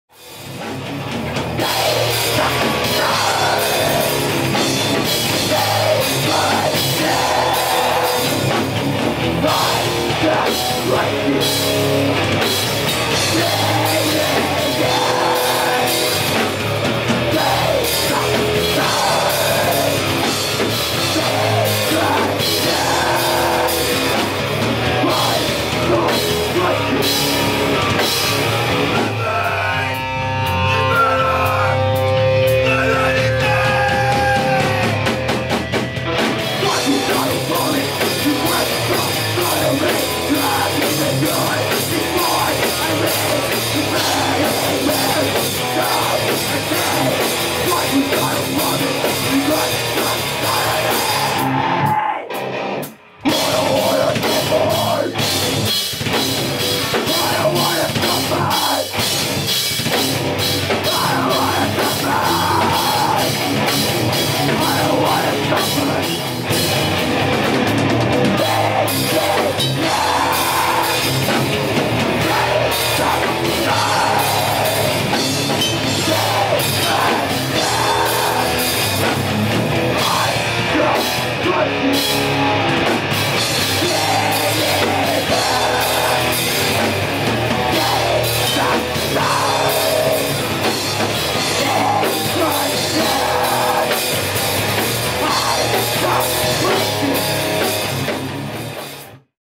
Mulholland
Ventura County Rock N Roll

Shot on 7/6/18 at Programme Skate & Sound in Fullerton, Ca.